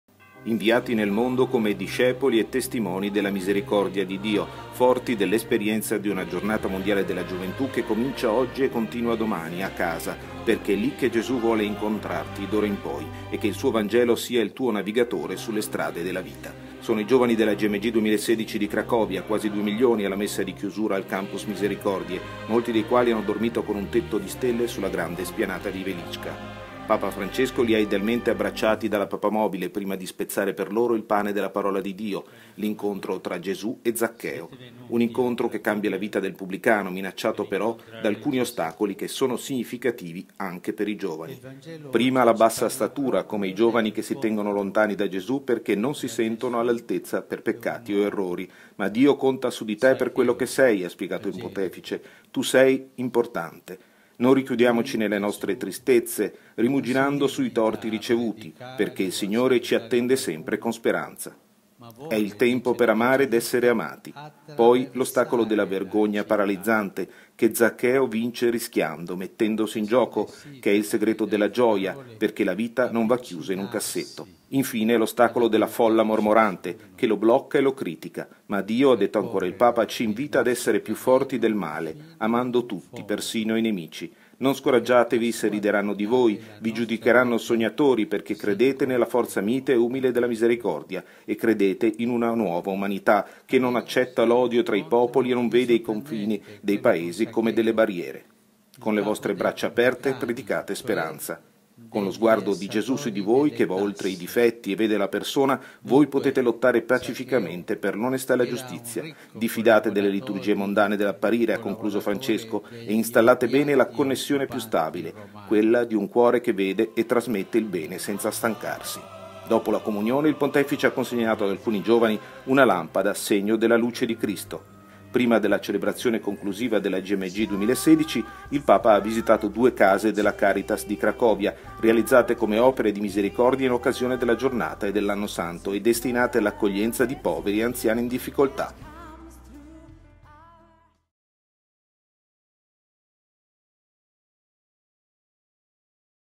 Papa in Polonia, messa chiusura Gmg 31.07.2016
Sognate un’umanità che non accetta l’odio tra i popoli

Nella messa di chiusura della Giornata mondiale della gioventù di Cracovia, Papa Francesco ha chiesto ai giovani di superare gli ostacoli all’incontro con Gesù, per costruire una nuova umanità “che non vede i confini dei paesi come delle barriere”. Dall’inviato Alessandro Di Bussolo.